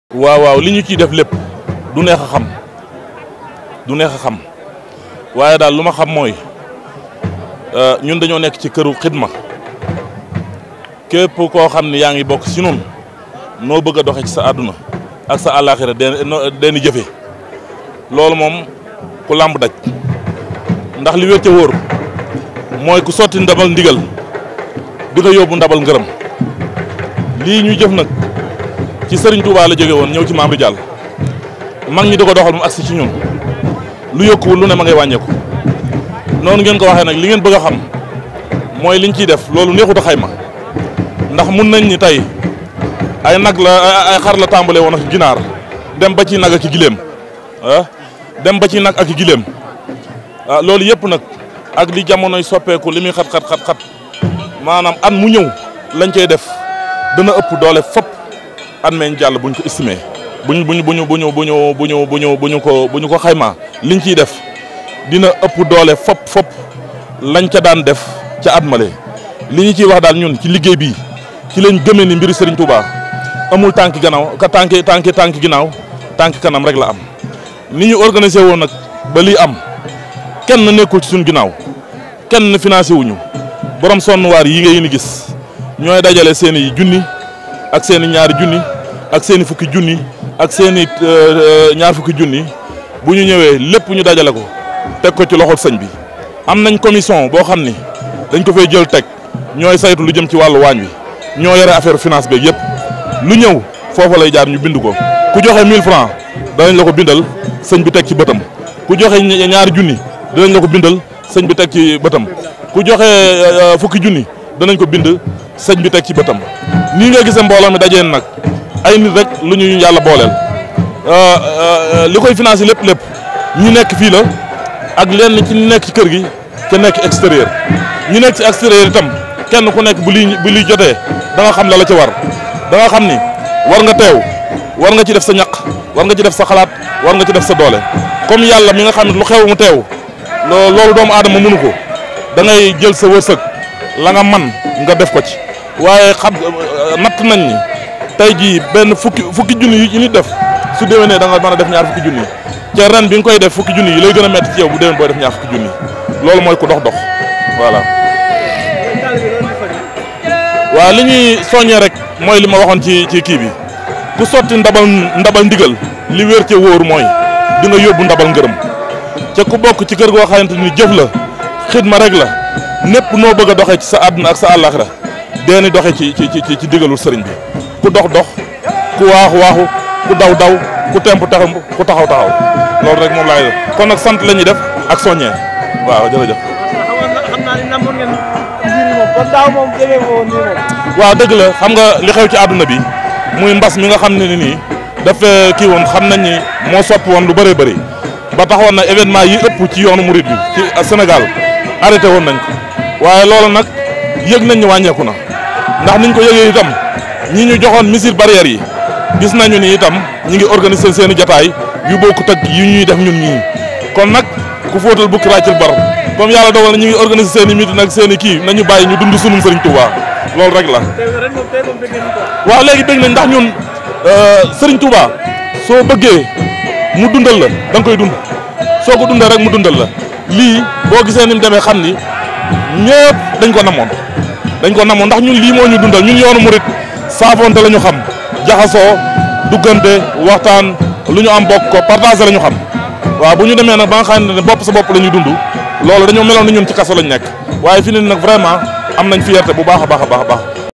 Wakeur Cheikh Ridial SECK : Serigne Amdy SECK Ridial Font le point sur les préparatifs Ramada 2021
Serigne Amdy SECK Ridial Font le point sur les préparatifs Ramada 2021